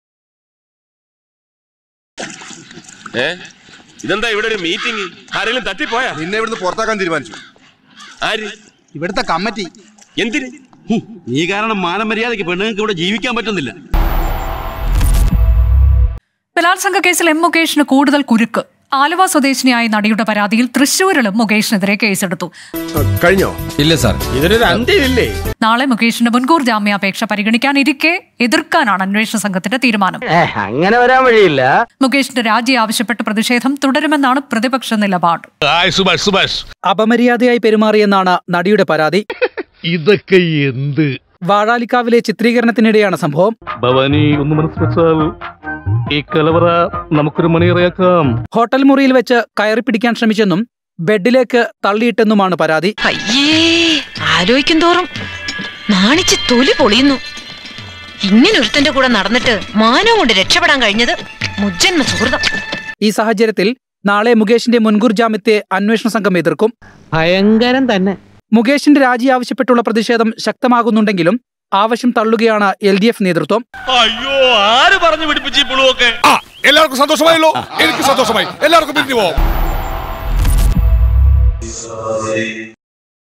മുകേഷിനെതിരെ വീണ്ടും പീഡന പരാതി ! Troll Video | Hema Committe Reort | Mukesh | Paathaalam Trolls
മുകേഷിനെതിരെ വീണ്ടും പീഡന പരാതി ! Troll Video | Rahul Mamkootathil v/s Mukesh | Paathaalam Trolls

COURTESY:
24 News

Channel Name : We Help Official (The Complete Entertainment Channel)  @WeHelpChannel